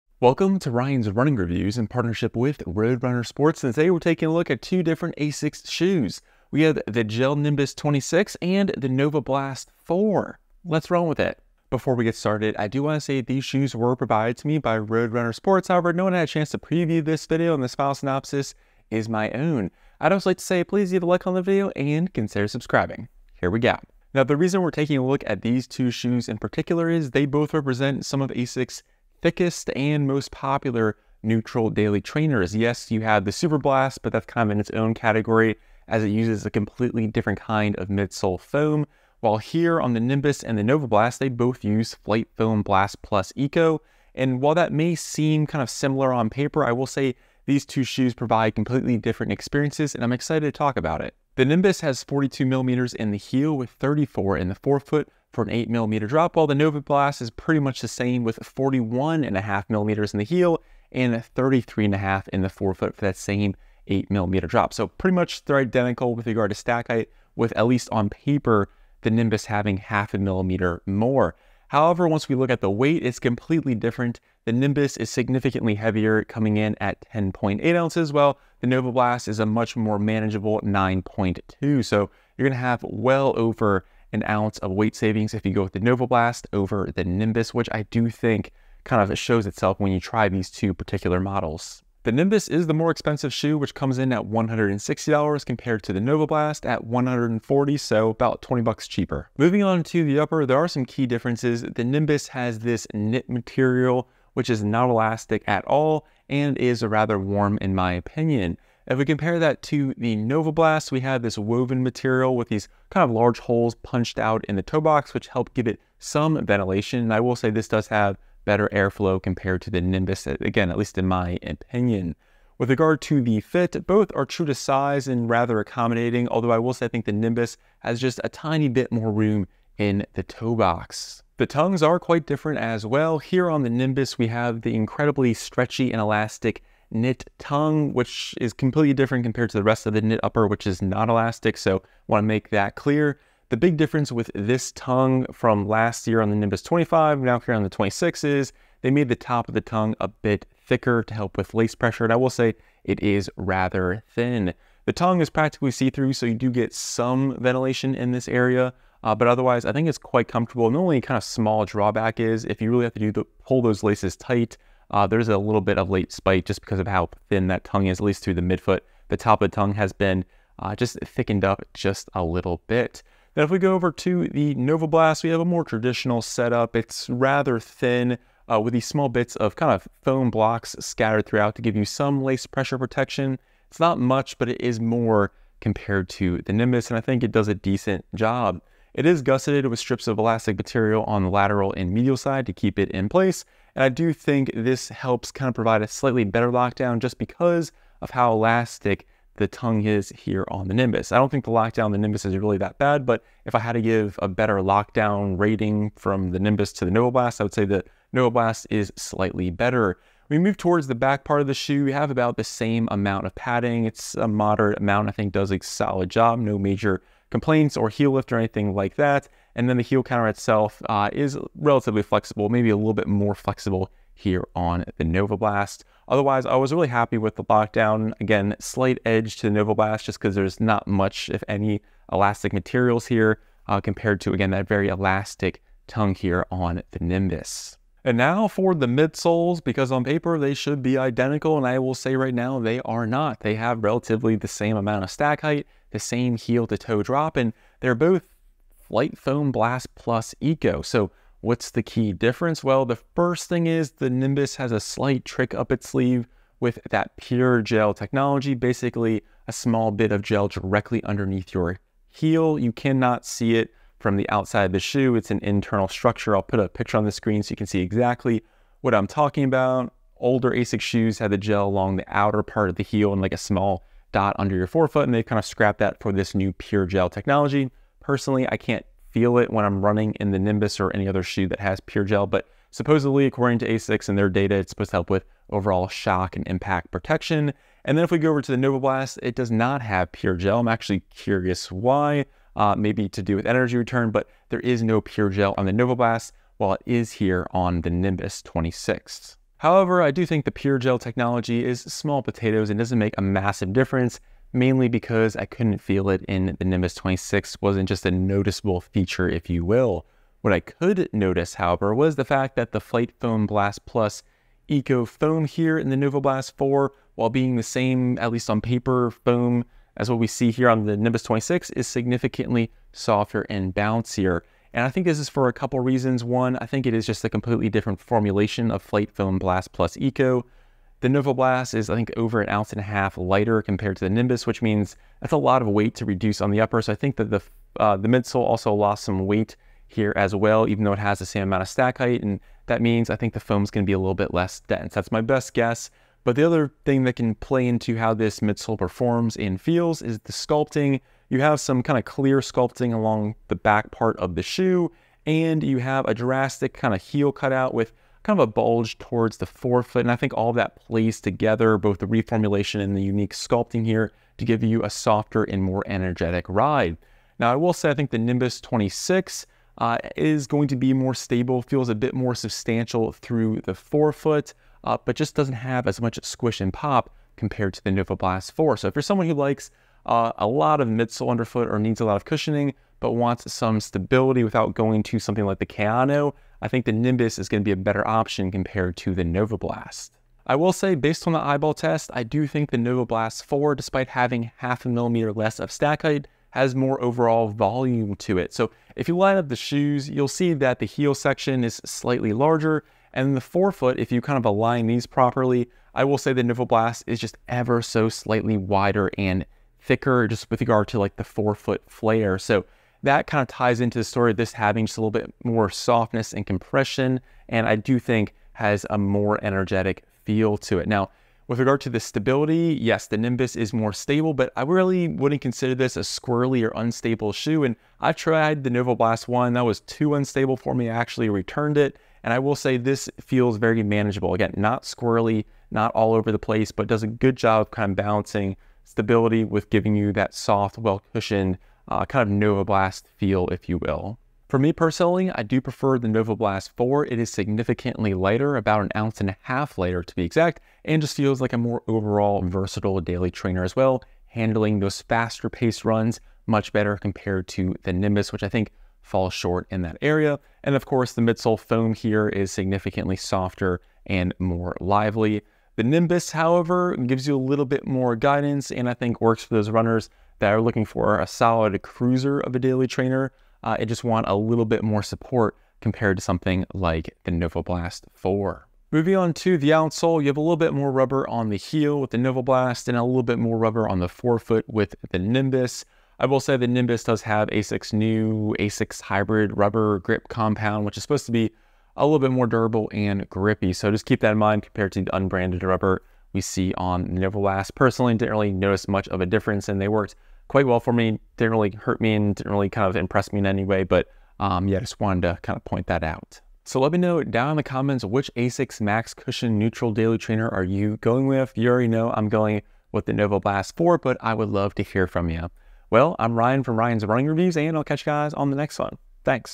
Energizing Updates to a Max Cushion Favorite! ASICS NOVABLAST 4 Review & Insights | RUN TEST REVIEW
🏃‍♂️ Today, we're looking at a max cushion favorite, ASICS NOVABLAST 4. Watch as we explore its unique design, share honest insights, and compare it to other ASICS models. Let's run through the details of the updated NOVABLAST 4! 👟